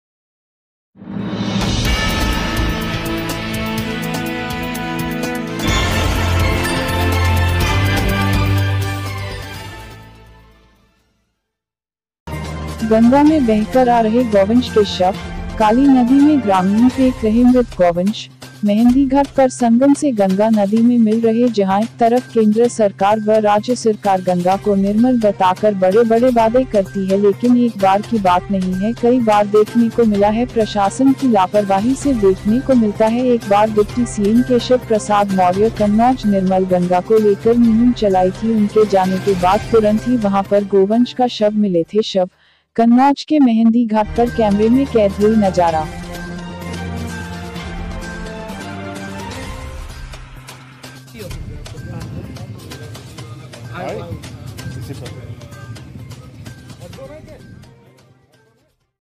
गंगा में बहकर आ रहे गौवंश के शव, काली नदी में ग्रामीण फेंक रहे मृत गौवंश
स्टेशन।  कन्नौज

ज्ञानेन्द्र दुबे कन्नौज उत्तरप्रदेश

कन्नौज ब्रेकिंग

गंगा में बहकर आ रहे गौवंश के शव, काली नदी में ग्रामीण फेंक रहे मृत गौवंश, मेहँदीघाट पर संगम से गंगा नदी में मिल रहे जहाँएक तरफ केन्द्र सरकार व राज्यसरकार गंगा को निर्मल बताकर बड़े बड़े ब्याडे करती है लेकिन एक बार की बात नही है कई बार देखने को मिला है प्रशासन  की लापरवाही से देखने को मिलता है एक बार डिप्टी सीएम केशव प्रसाद मौर्य कन्नौज निर्मल गंगा को लेकर मुहिम चलाई थी उनके जाने के बाद तुरंत ही वहां पर गोवंश का शव मिले थे शव, कन्नौज के मेहँदीघाट पर कैमरे में कैद हुए नजारा।